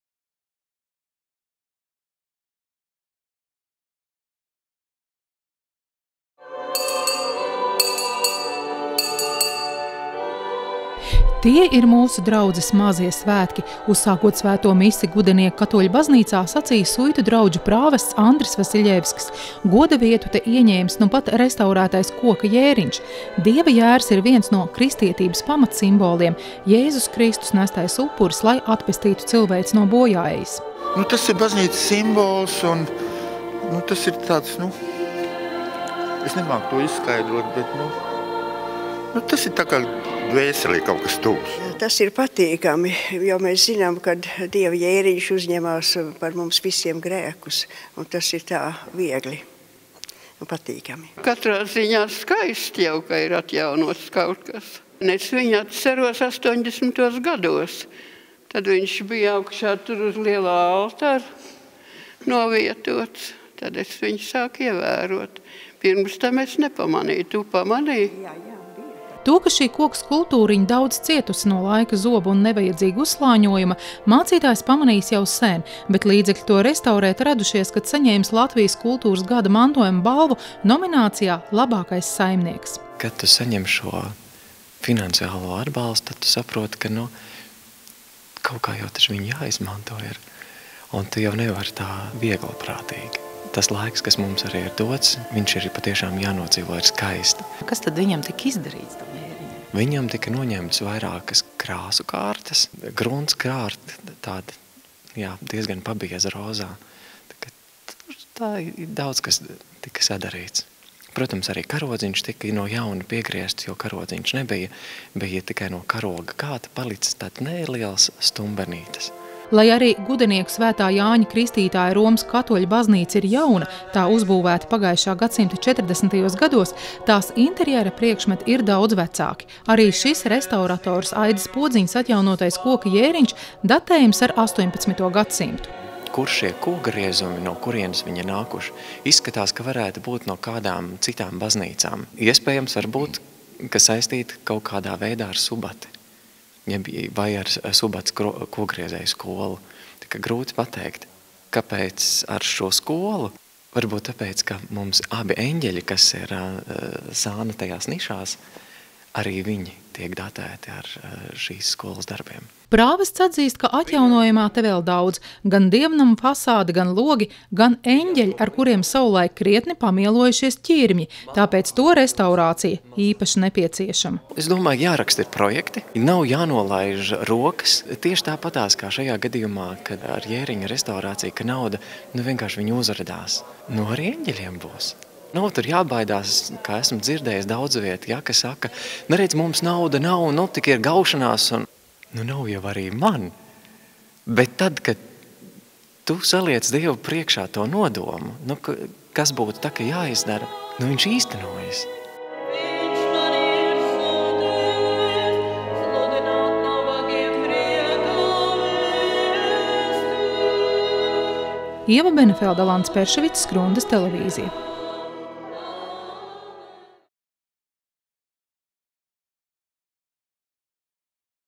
Gudenieku baznīcā atgriežas koka jēriņš
Šie Ziemassvētki Gudenieku baznīcā citādāki nekā ierasts. Neilgi pirms svētkiem savu sākotnējo izskatu ieguvusi viena no koka skulptūrām -- jēriņš. Tā restaurācijā draudzes priesteris Andris Vasiļevskis ieguldījis saņemto Kultūras gada mantojuma apbalvojuma līdzekļus.